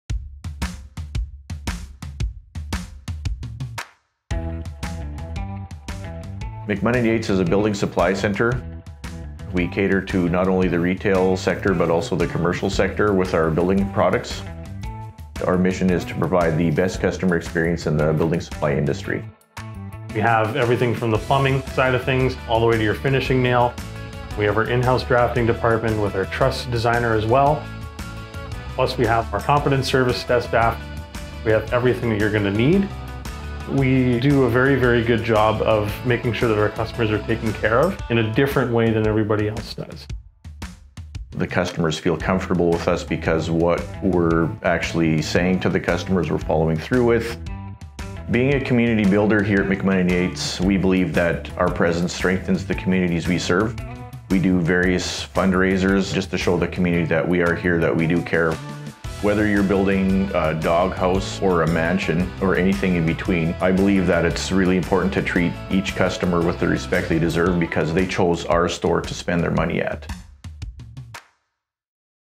Welcome to McMunn & Yates in Steinbach, Manitoba!
Discover a new shopping experience at our renovated Steinbach location! Enjoy expanded product selection and dedicated Contractor Services, including in-house drafting, flooring designs and installs, concrete services and so much more! We're ready to serve you.